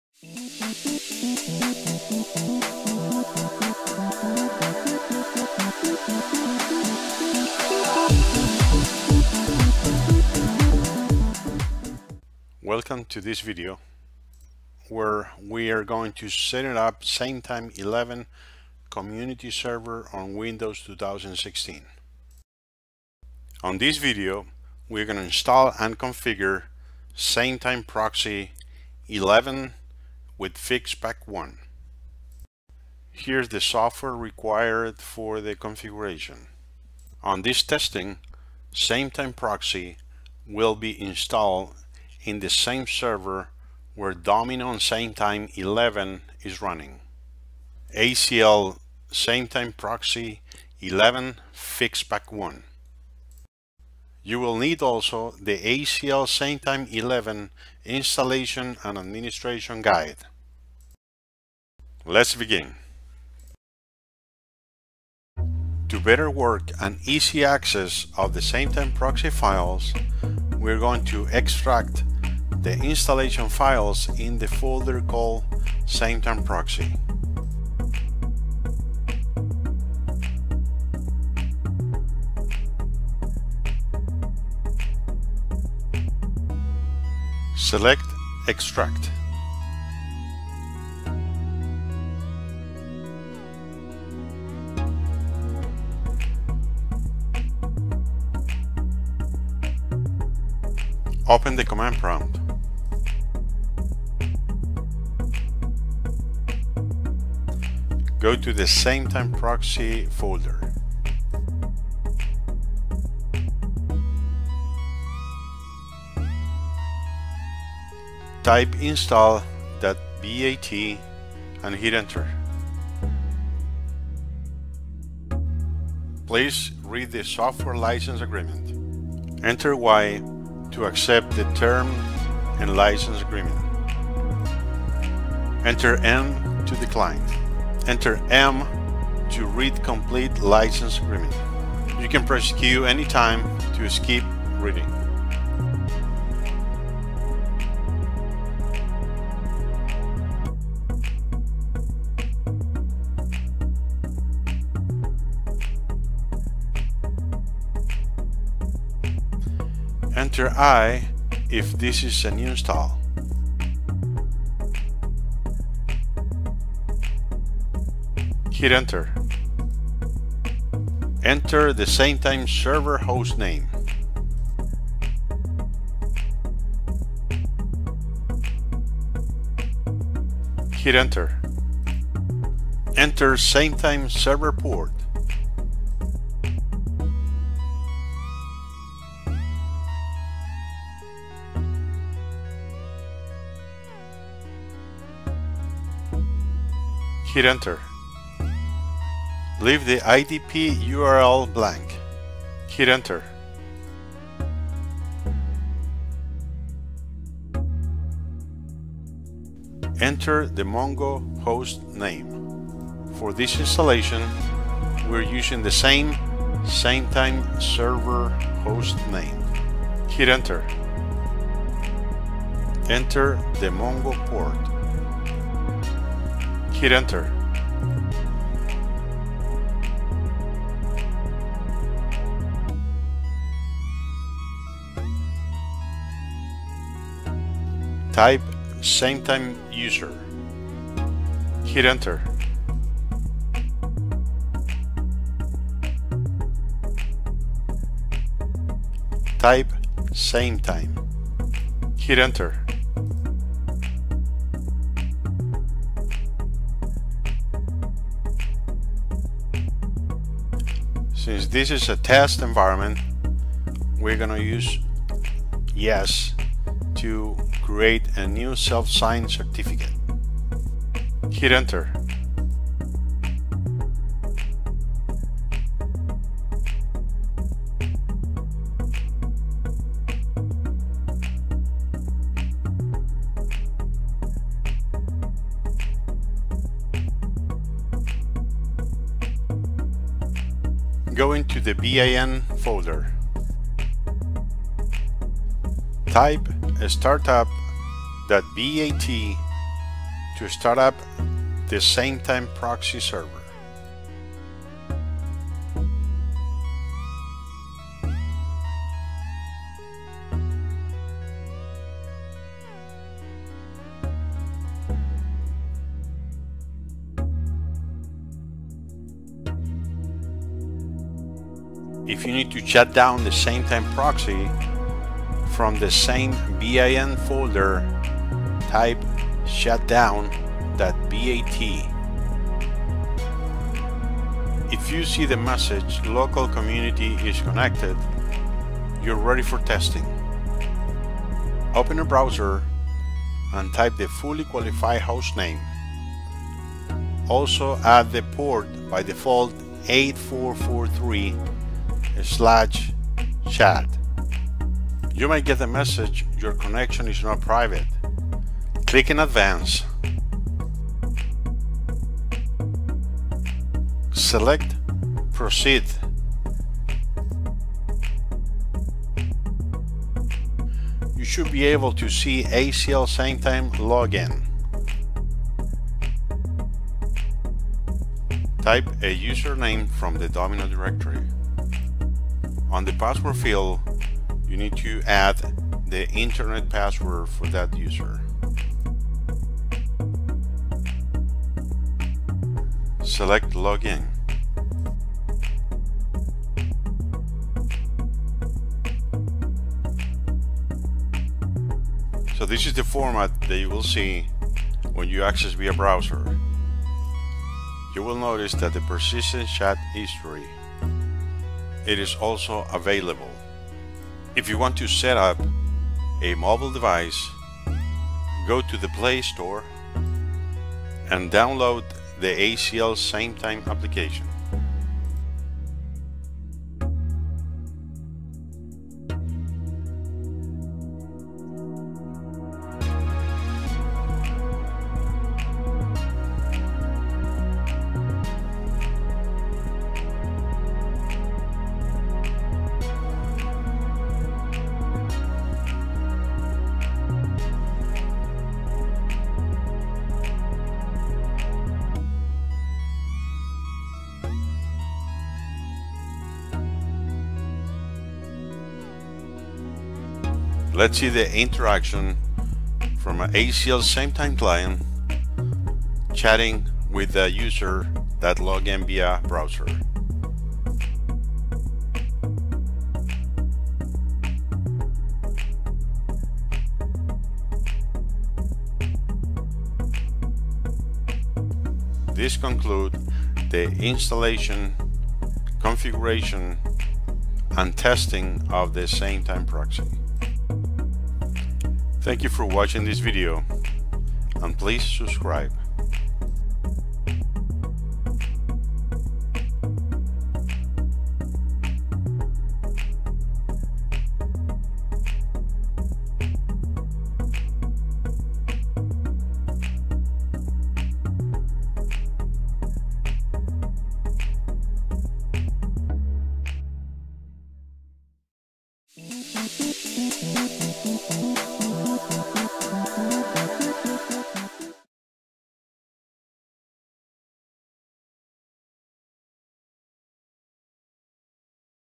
Sametime 11 Install on Windows 2016 (Part 5)
This is the last part of a five video series to help you implement a Sametime 11 FP1 Community Server in Windows 2016. This video will help you to set up Sametime Proxy 11 FP1.